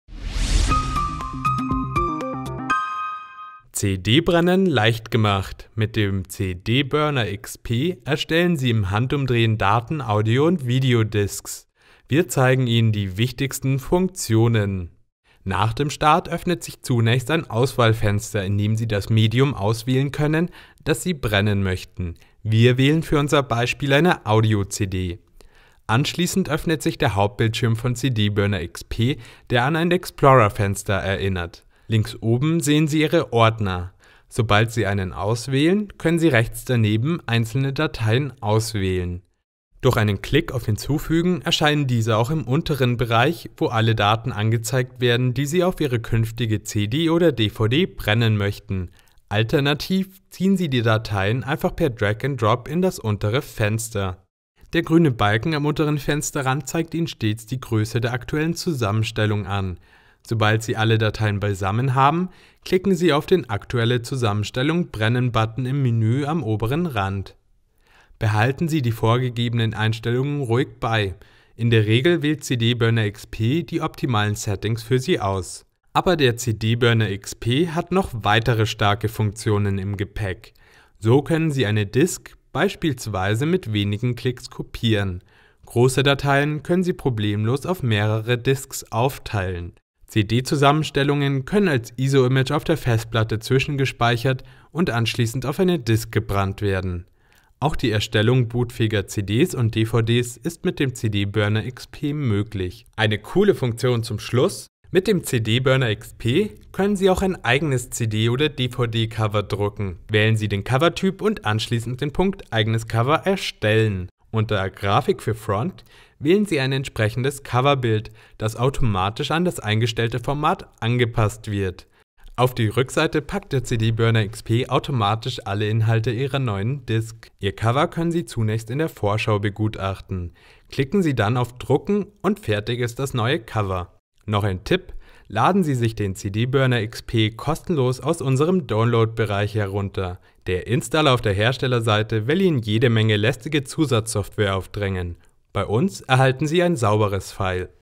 CDBurnerXP - Kostenloses Brennprogramm | CHIP
Mit dem kostenlosen CDBurnerXP erstellen Sie im Handumdrehen Daten-, Audio- und Videodisks. Brennen Sie mit wenigen Klicks CDs, DVDs und Blu-rays. Wir zeigen Ihnen die wichtigsten Funktionen.

Haben sie Fragen oder Probleme mit einer Software? Im CHIP Forum wird ihnen geholfen